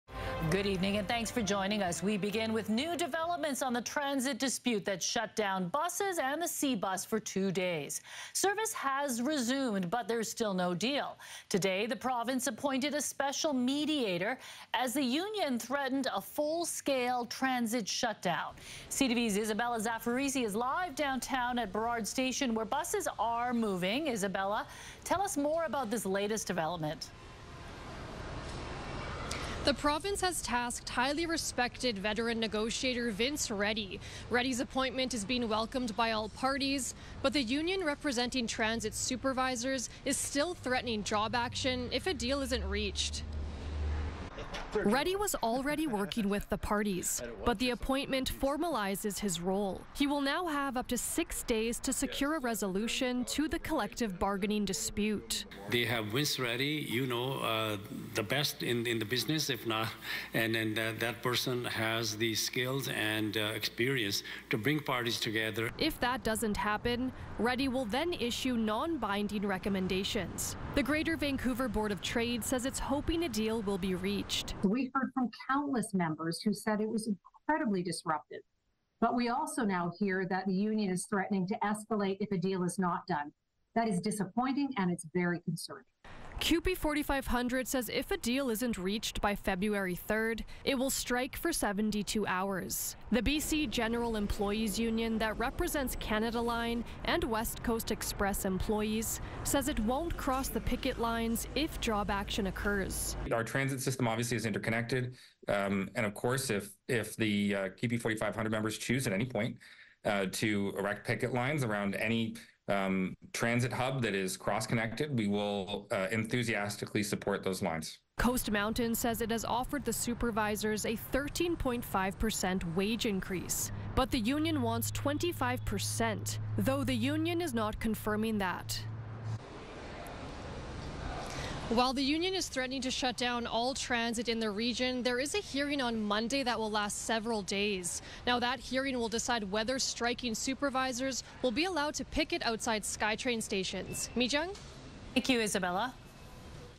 Union threatens full Metro Vancouver transit shutdown
Bus and SeaBus service has resumed, but there’s still no deal. The province appointed a special mediator Wednesday.